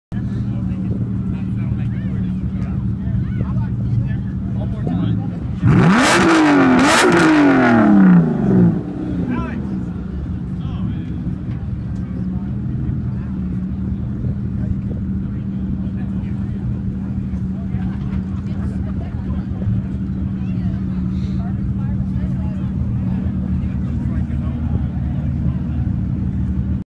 Aston Martin Lagonda Limited Edition V10 Revs
Seen at cars & coffee dallas